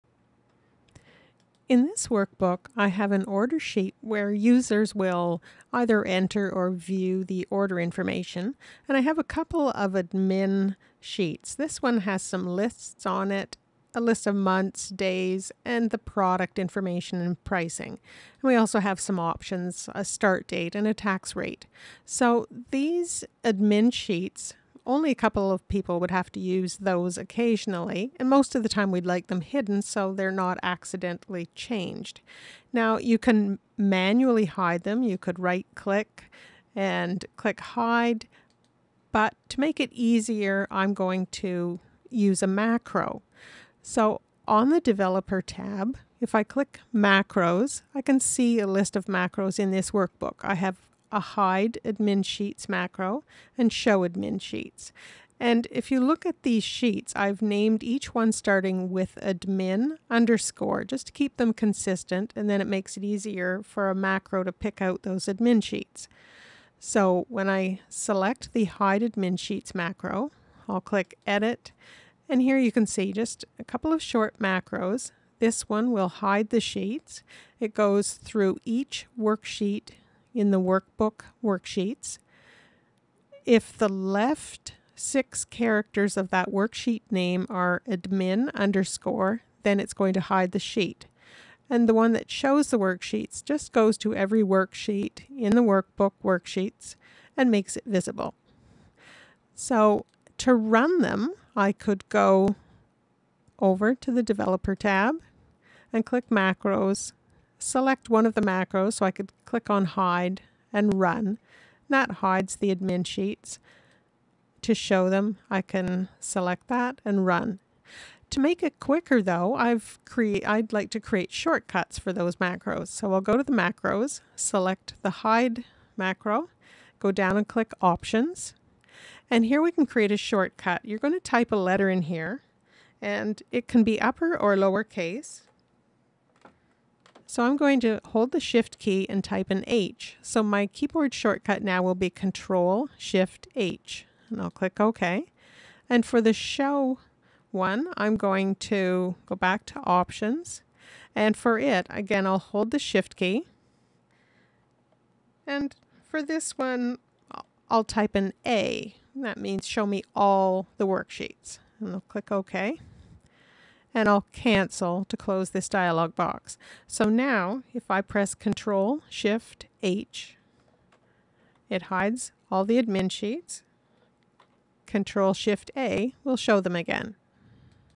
Hide Specific Excel Sheets With a Macro Shortcut
Use a macro to show or hide the Admin sheets in your Excel workbook, so users don't accidentally change the data.